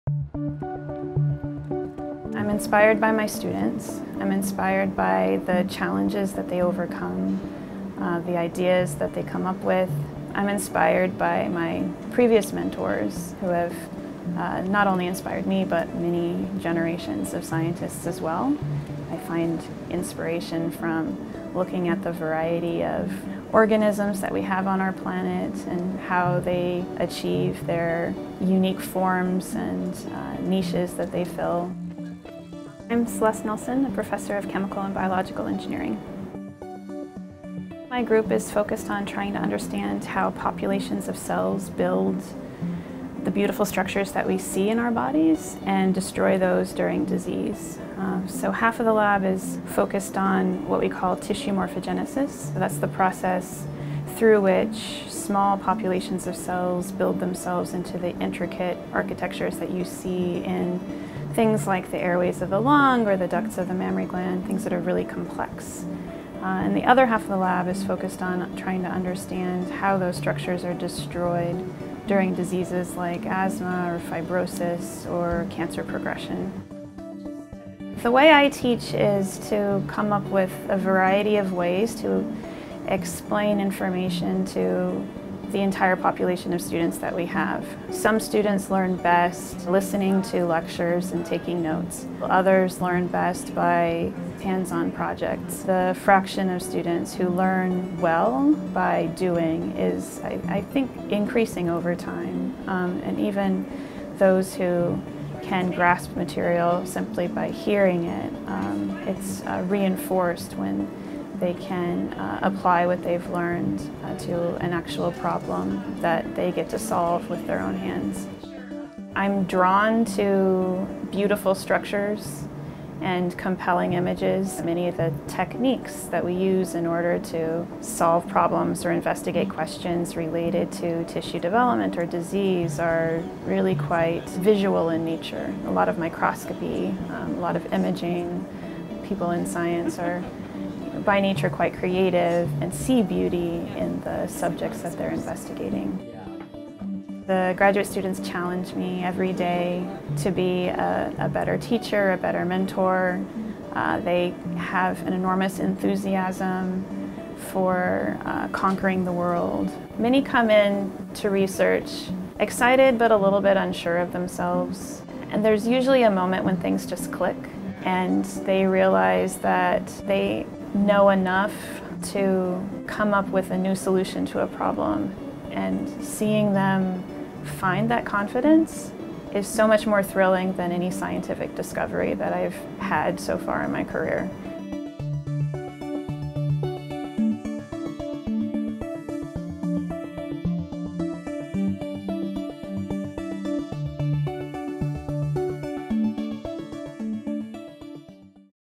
Princeton Profiles: Celeste Nelson, inspired by her students and the beauty of science
Celeste Nelson, a professor of chemical and biological engineering who studies how tissues and organs develop, finds beauty in science, passion in teaching and purpose in mentoring. (Video by Danielle Alio, Office of Communications)